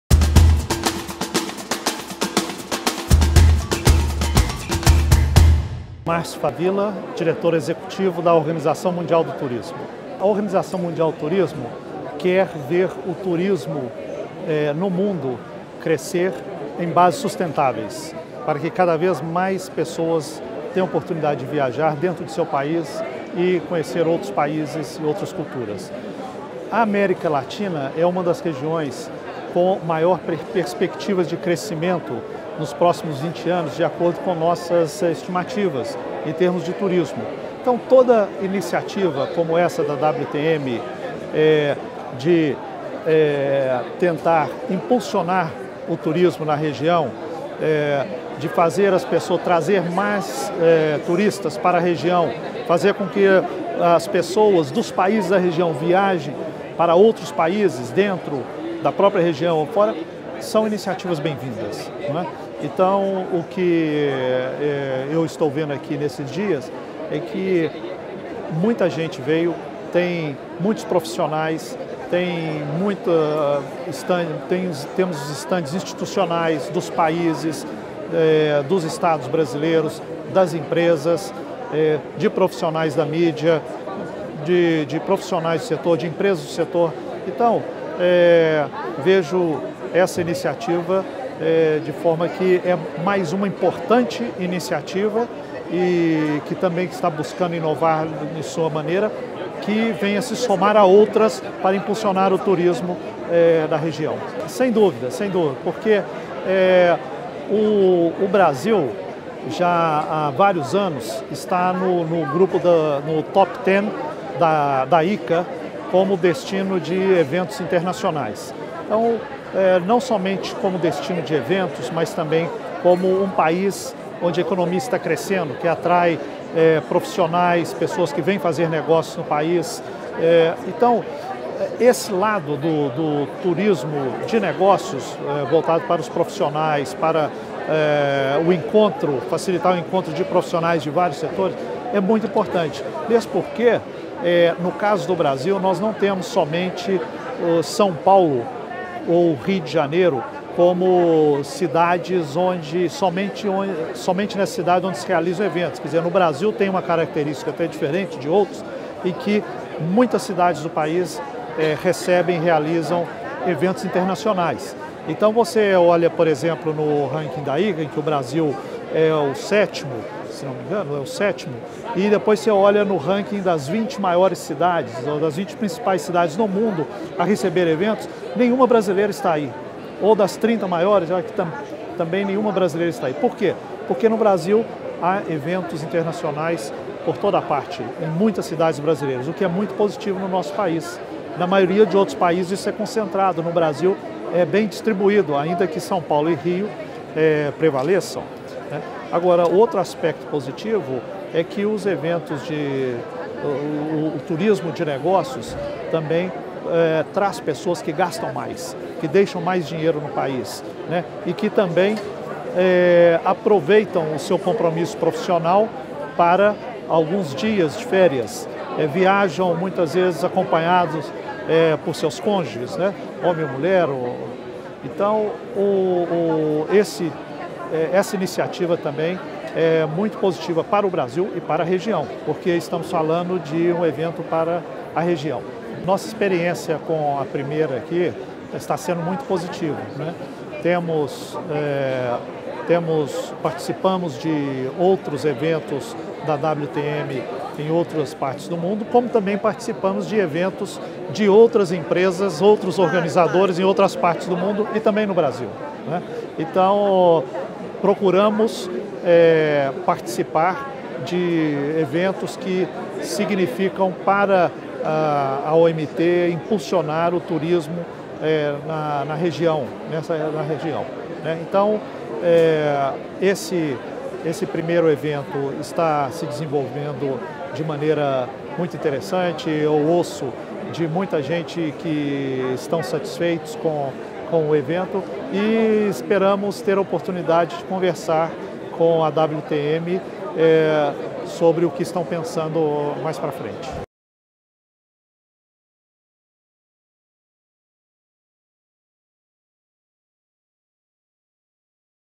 WTM Latin America 2013 Interview World Tourism Organization
Márcio Favilla, Executive Director of the World Tourism Organization.